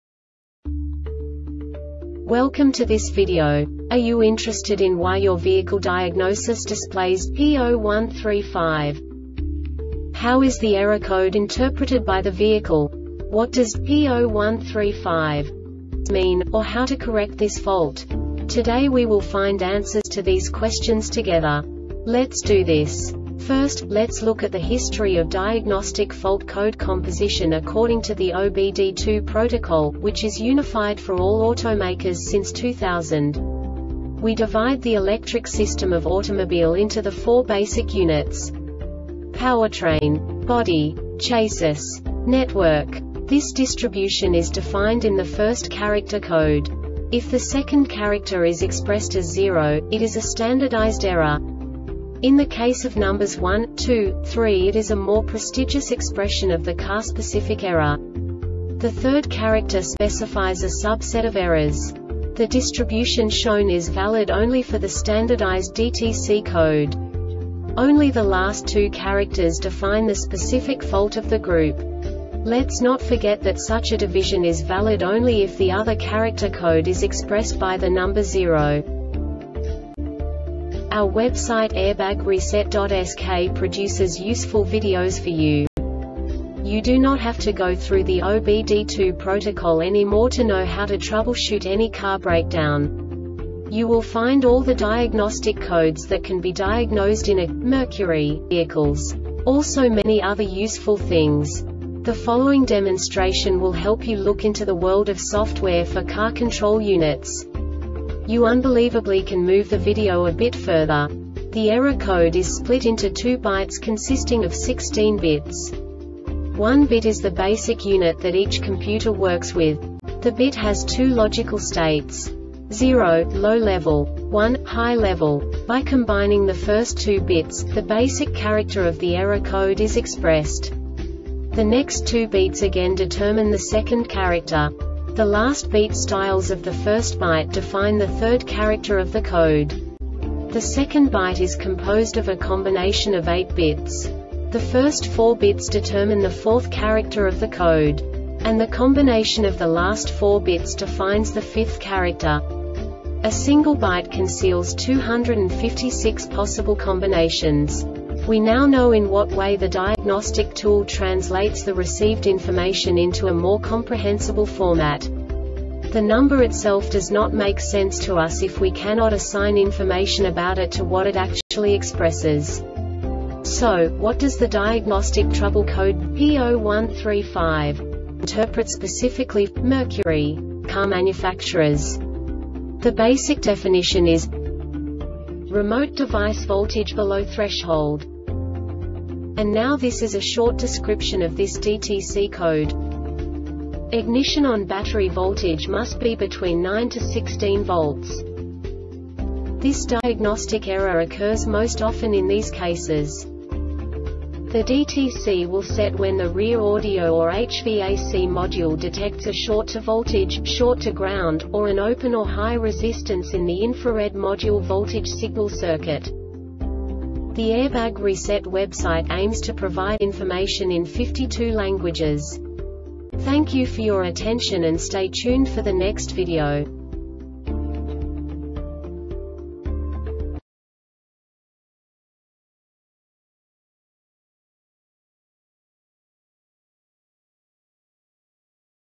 DTC Mercury P0135 Short Explanation
Contents:
0:21 Basic DTC analysis according to OBD2 protocol standard.
1:48 Insight into programming
2:58 A diagnostically understandable description of the Diagnostic Trouble Code
3:05 Basic error definition

Make
Mercury
Code
P0135
Definition
Remote Device Voltage Below Threshold
Description
Ignition ON  Battery voltage must be between 9-16 volts
Cause
The DTC will set when the rear audio or HVAC module detects a short to voltage,  short to ground,  or an open or high resistance in the infrared module voltage signal circuit